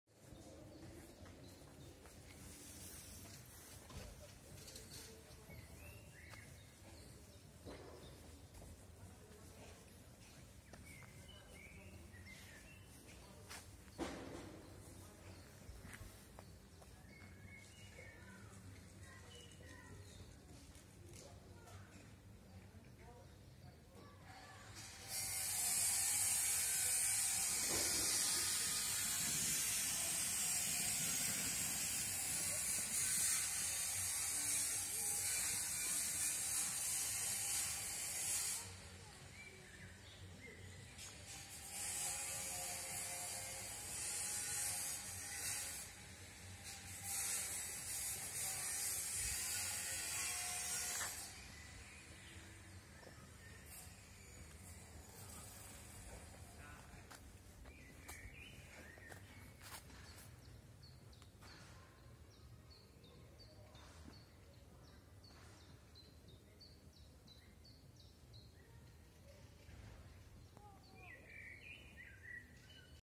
У Трускавці руйнують старовинну вілу Великий Косинір 13 травня 2020 р.
У Трускавці руйнують старовинну вілу Великий Косинір